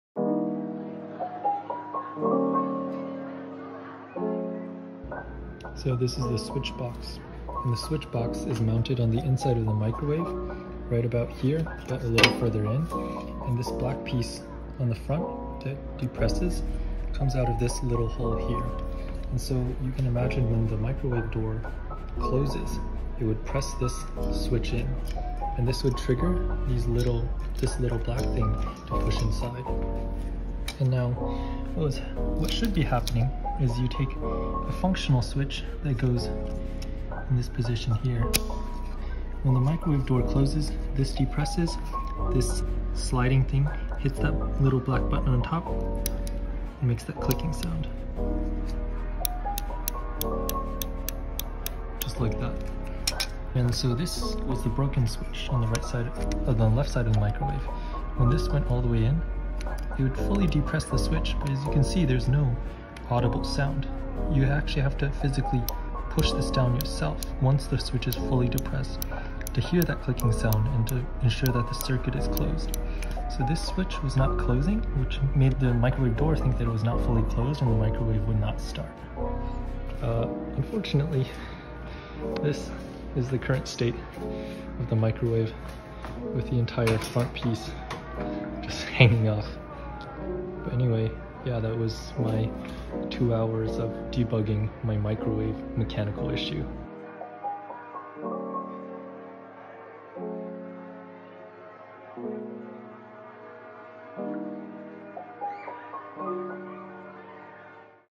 How I Accidentally Fixed (and destroyed) my Microwave | see description for full story
“Read more” for FULL story

A few days ago, my microwave stopped working. When you clicked the start button, it would start whirring for a split second before immediately stopping.

This was a huge issue because the microwave is part of an integrated microwave-oven combo, and fixing it would cost $600 or replacing it would cost $3000.

Thus, I decided to try and fix the issue using the limited knowledge I have. My first instinct was the door closing mechanism for a few reasons. Firstly, the microwave had been functioning properly just a few days ago, and still started up for a split second. This meant that the heating and operations were likely still functional. Secondly, we’ve had small issues with having to repose the microwave door twice before it would start sporadically in the past. I hypothesized that there was some mechanism that triggered when the microwave door closed that would indicate it was safe to operate.

More research showed that there were two mechanical buttons on the inside of the microwave door that were depressed when the door closed. These buttons triggered 3 separate switches, one on the left and two on the right side of the microwave. Each of these switches served different purposes, but all were related to ensuring the door was closed while the microwave was operating.

The tear down process to actually find the switch box involved remixing most of the paneling around the back of the microwave. This was a pain at points because the screws used were incredibly small hex flower shaped. Luckily, from previous repairs of my entire MacBook screen in the past, I just happened to have a screwdriver that would undo these screws.

To diagnose the issue with the switches, i began by reading the entire spec sheet for the switches.

For the switches, I began by testing for continuity using a multimeter, and all switches passed the test. However, I noticed that one needed to be pressed d
own significantly more to make the clicking sound than the other two. Pulling out the switch housing confirmed my suspicions that there was indeed a mechanical issue with the switch, as shown in the video.

Because of this discovery, my family was able to buy a set of 3 switches for $8 instead of spending more than $600. Hopefully the cadmium coated terminals of the switches don’t come back to haunt me.